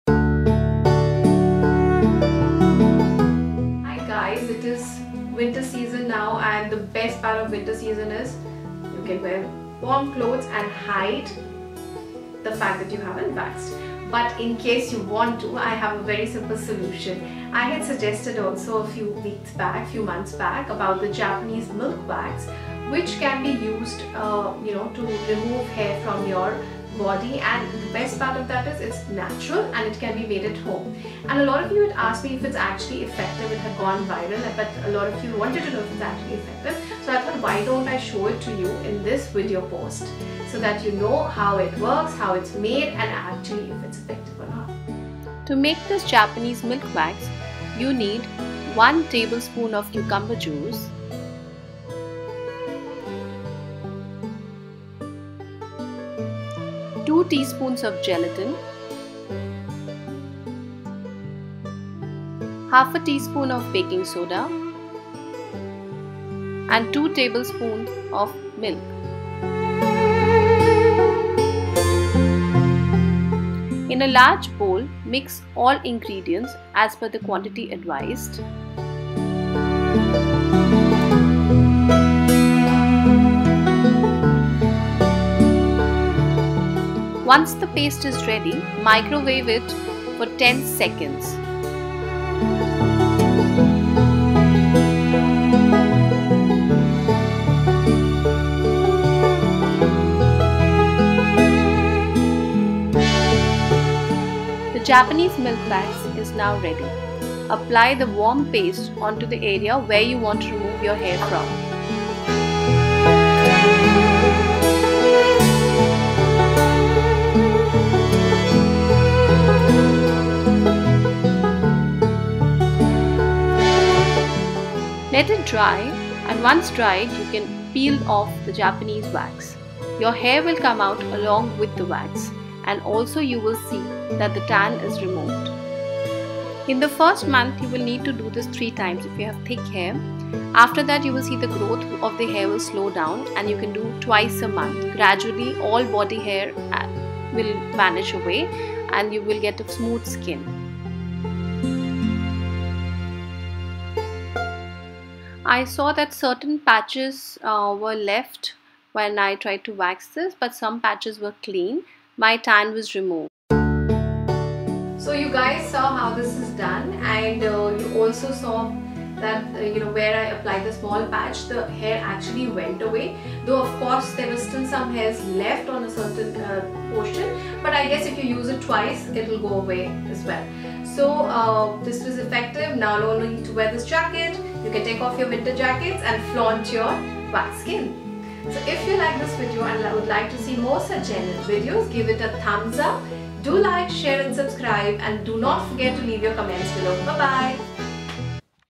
Find of the week Japanese Milk Wax
Body hair is such a nightmare for a woman. Waxing is the only way to remove the hair but at a salon, the procedures chosen are expensive and some of us are sensitive to the wax used and even worried about hygiene. What if you prepare your own homemade hair removal wax? It will naturally remove all the unwanted hair and make your skin soft and hair-free. Here we are introducing the concept of milk wax and
sharing how you can make it.

Welcome to the MOABFamily!! Hope you liked the video, do let us know!!